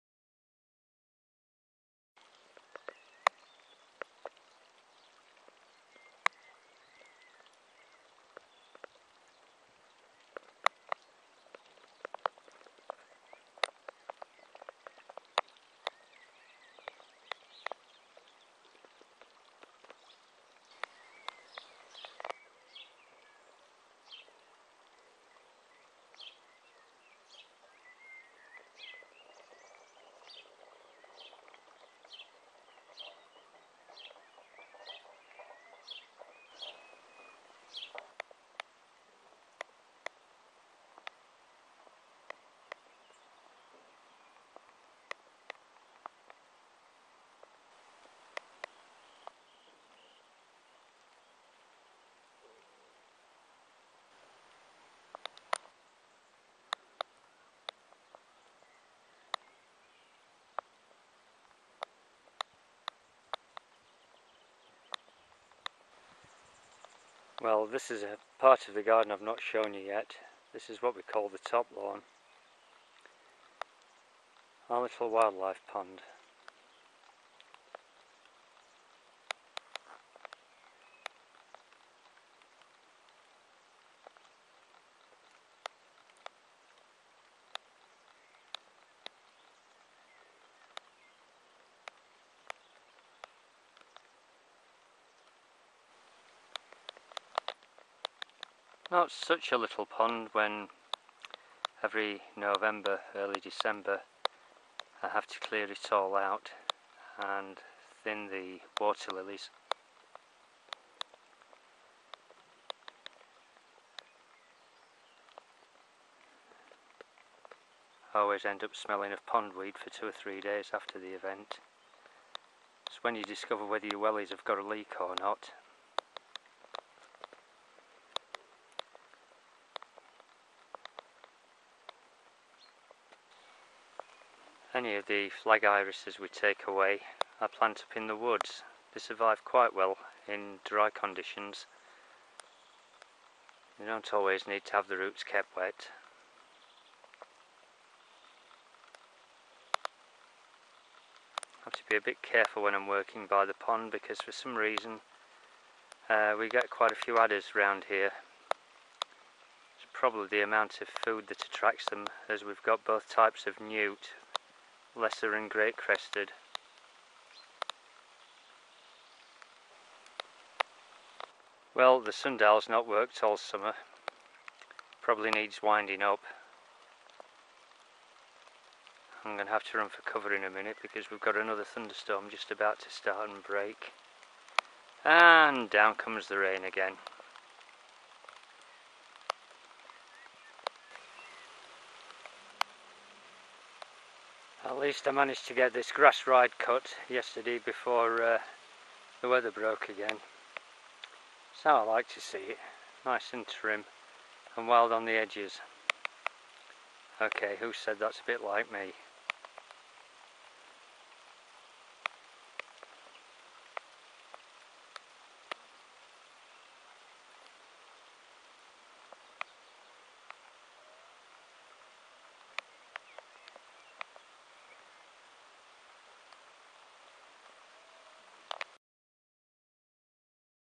Hooray Hall High Summer 3
Hooray Hall Gardens and the upper Lawn. High Summer you must be Joking.
This was shot the day before the West Country Floods.